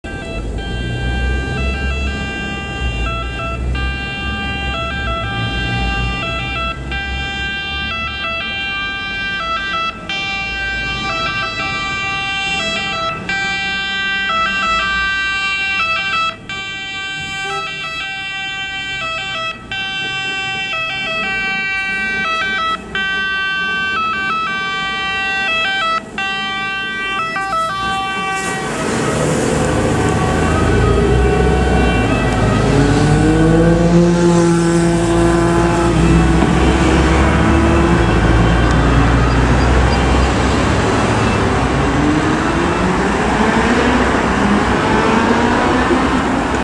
Autogrù Astra Cormach VVF di Ancona in Emergenza / Italian Fire Brigade Crane Truck in Emergency
Un'autogrù Astra HD7 84.42 Cormach dei vigili del fuoco di ancona transita in emergenza con sirena e lampeggianti accesi.
Ancona, Marche, Italia, Maggio 2018.
(ENGLISH)
A crane truck of the italian fire brigade of Ancona transit in emergency with siren and flashing lights actived.
The truck is an Astra HD7 84.42 setup by italian company "Cormach".
Ancona, Marche, Italy, May 2018.